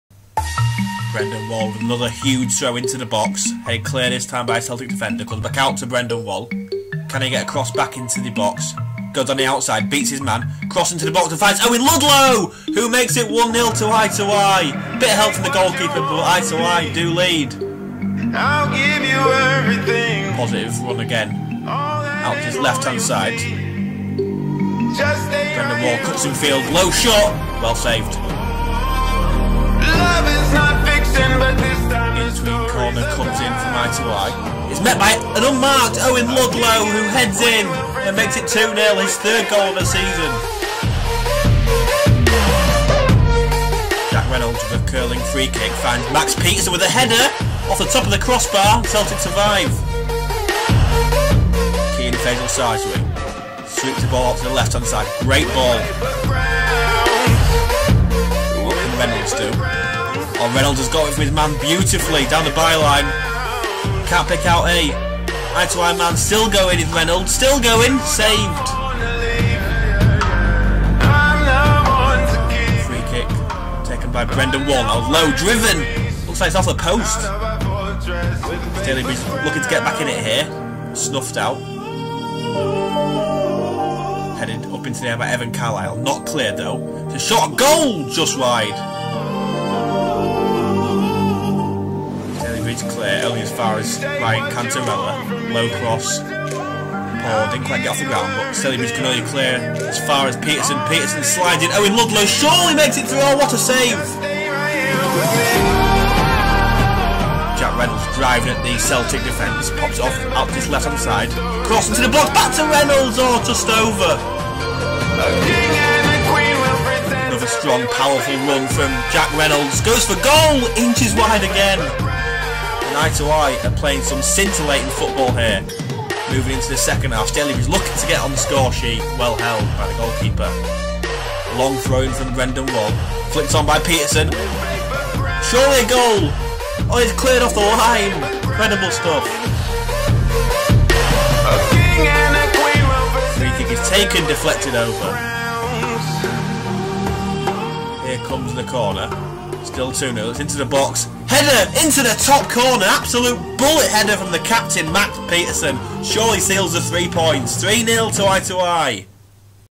Highlights: i2i Blue vs Stalybridge Celtic
Jono Greening's blue team made it two wins out of three in the Lancashire league with another fine display at York St John University.